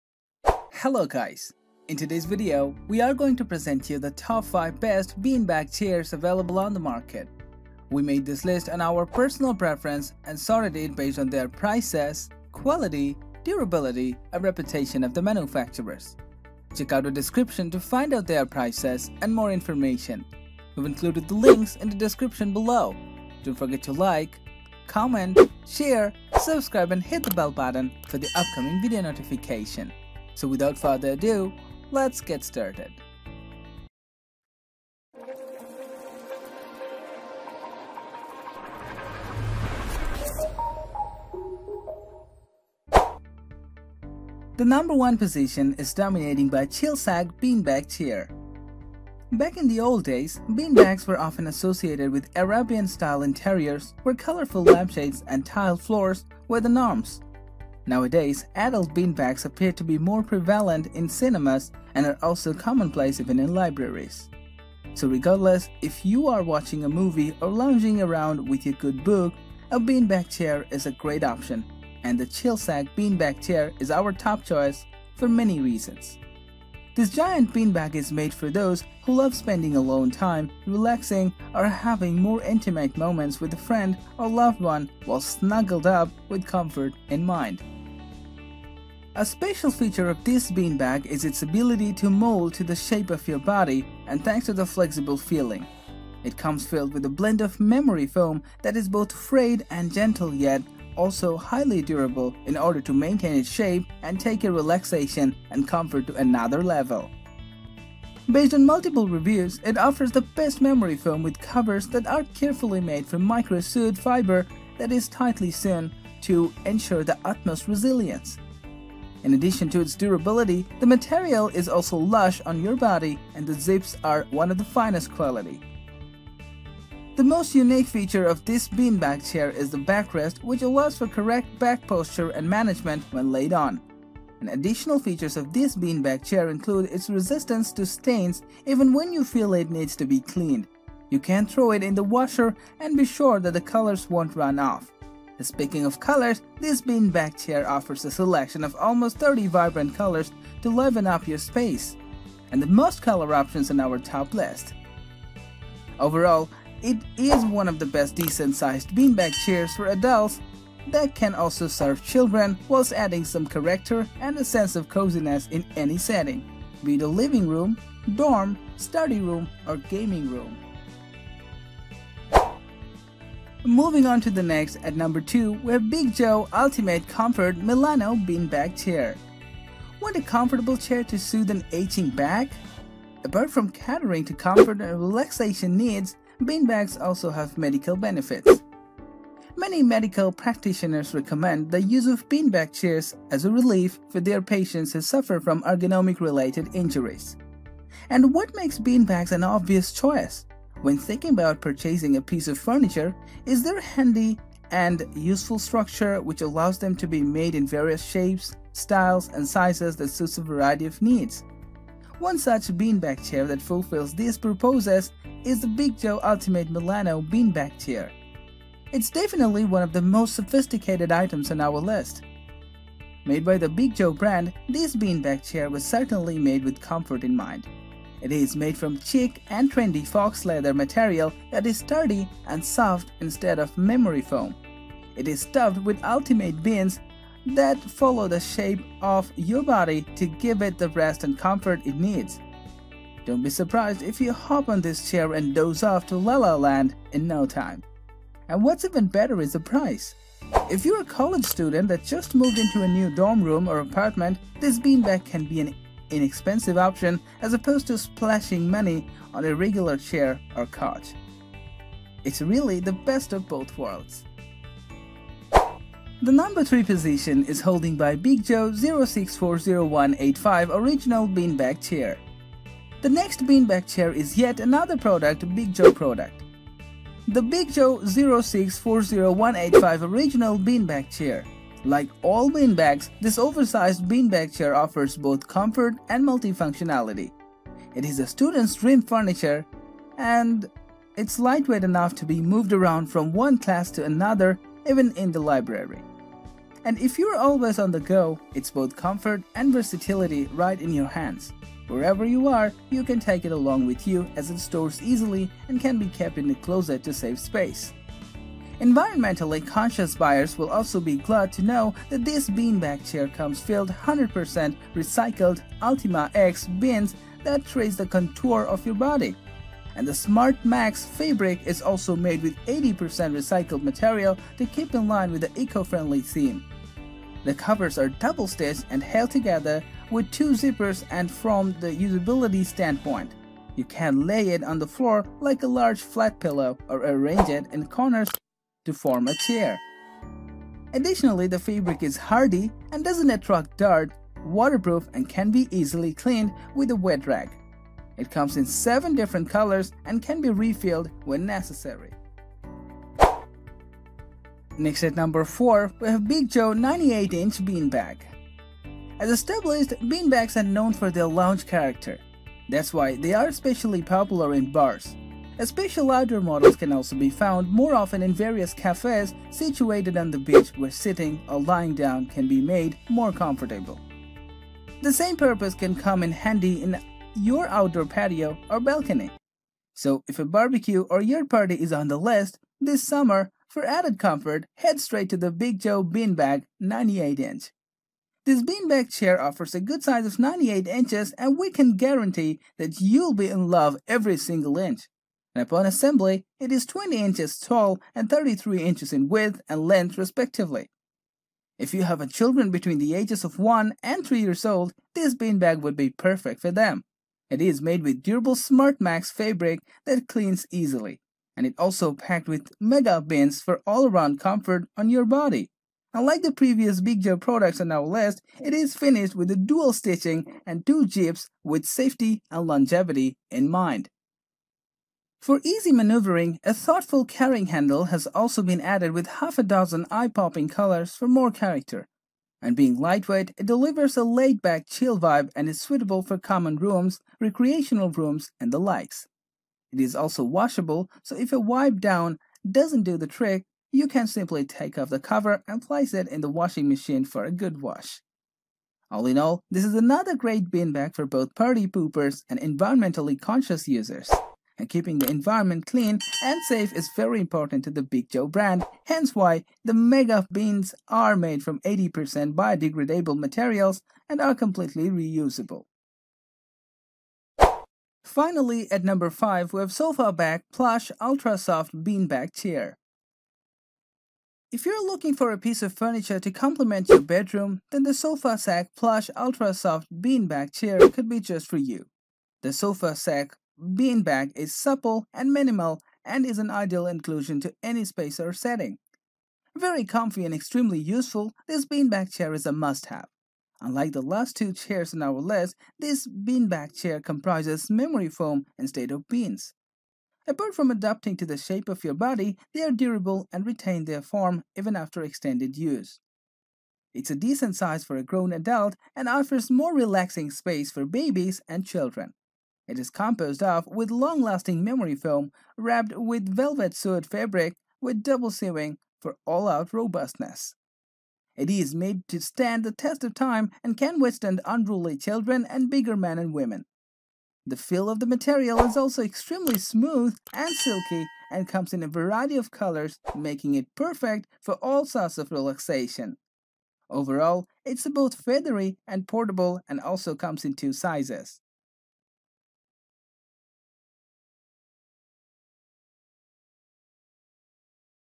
Best Bean Bag Chair Reviews 2024 | Best Budget Bean Bag Chairs (Buying Guide)
Are you searching for the Bean Bag Chairs of 2024? These are the Top 5 Bean Bag Chairs we found so far:

1. Chill Sack Bean Bag Chair:

2. Big Joe Lumin Bean Bag Chair:

3. Big Joe Original Bean Bag Chair:

4. Big Joe Bean Bag  Chair:

5. Sofa Sack - Plush, Ultra Soft Bean Bag Chair:

To narrow down your searching effort, we have researched the market on the Bean Bag Chairs. We already spent hours analyzing these top 5 Bean Bag Chairs to ensure your worth buying.They all come with excellent features with a great price range.

This video on the Bean Bag Chairs reviews in 2024 will add value for the money. So keep watching till the end and select the suited one for you.